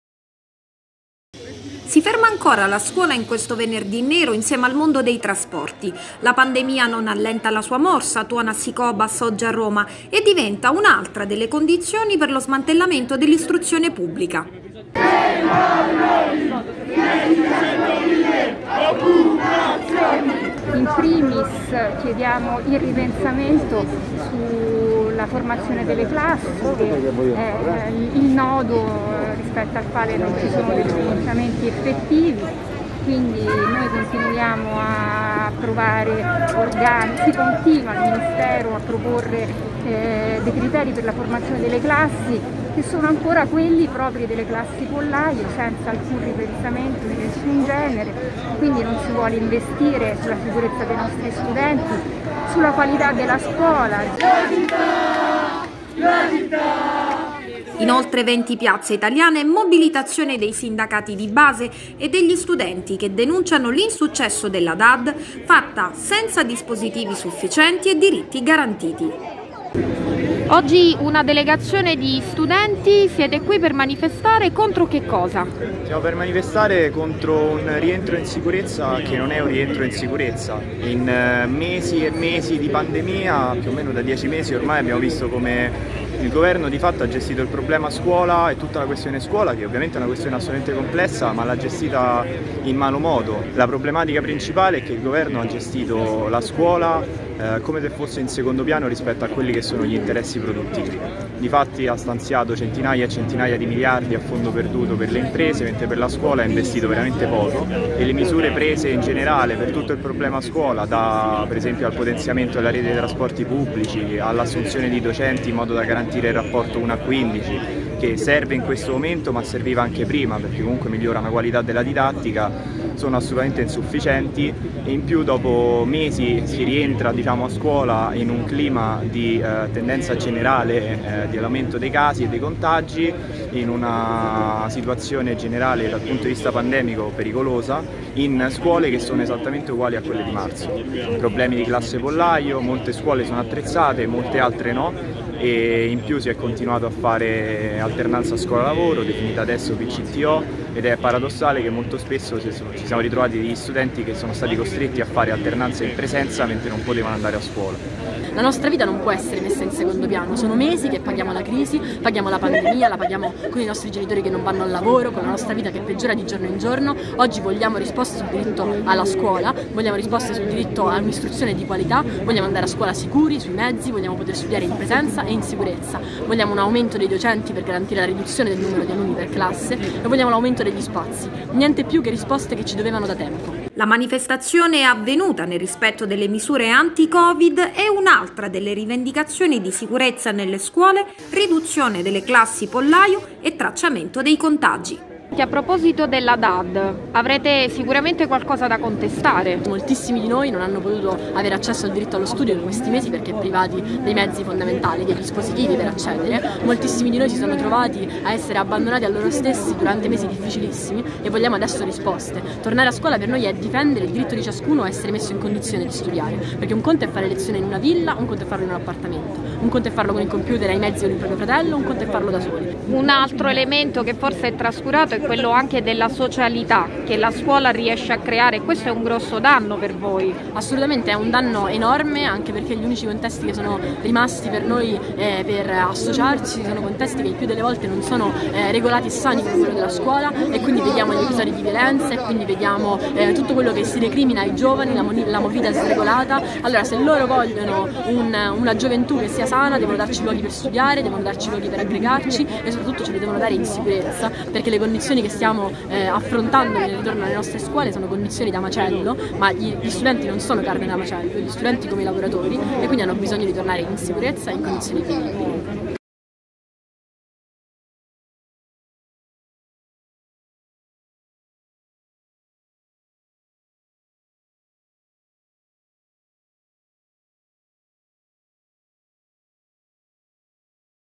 Gli studenti in piazza contro il governo: "ci hanno abbandonato"
Manifestazione di studenti a Roma riguardo i precari, la didattica a distanza e le classi pollaio.
Servizio a cura di Jessica Saccone